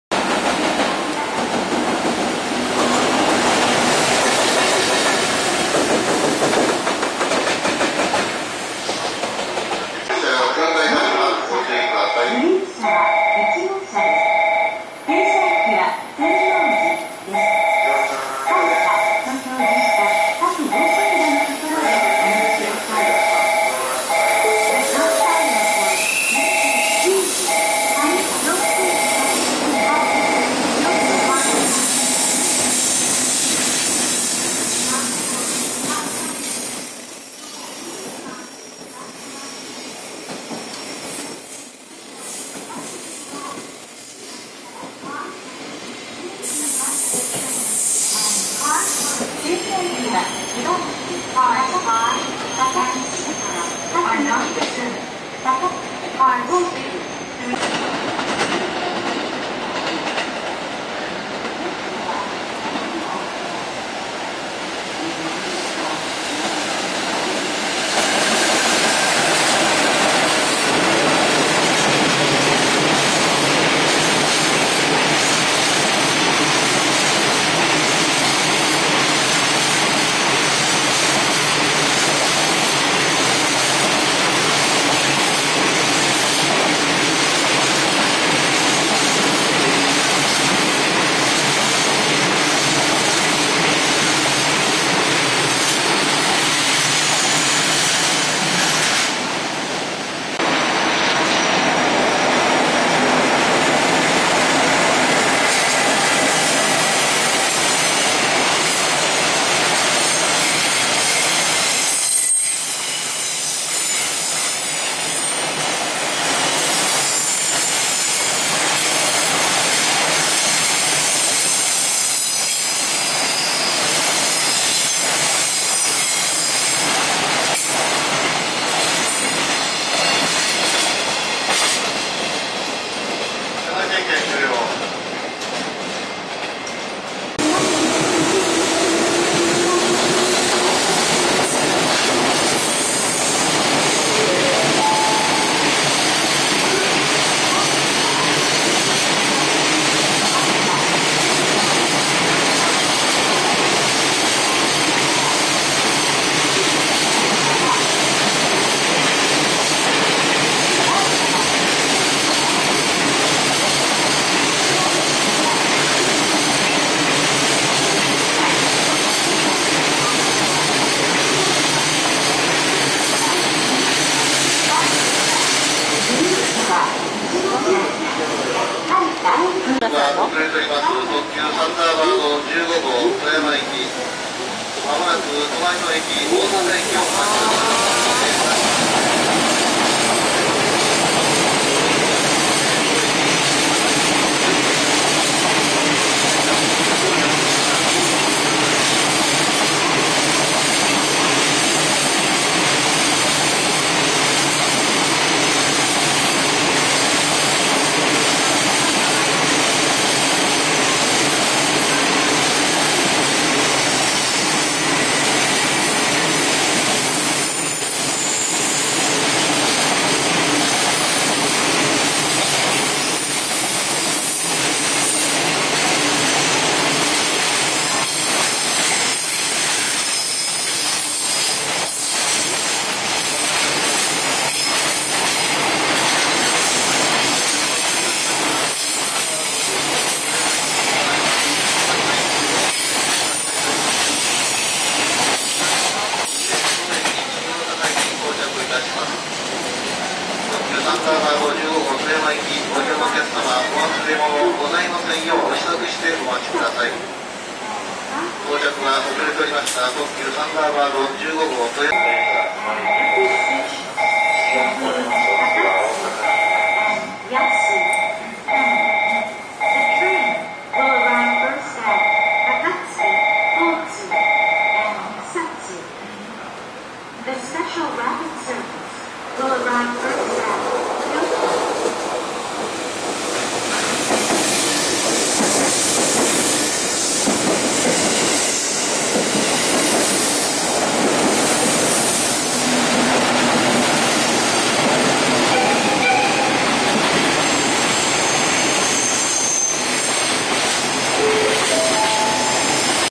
[FHD]新大阪を通過する列車いろいろ(20130202 1000-1110) Trains@Shin-Osaka station
新大阪を通過する列車いろいろ(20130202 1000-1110) Trains@Shin-Osaka station
単機/臨時列車回送/無動付き貨物列車/変わったコンテナ など Single Locomotive,Extra train Deadhead,Freight w/deadheading Loco,Rare Freight Container,etc.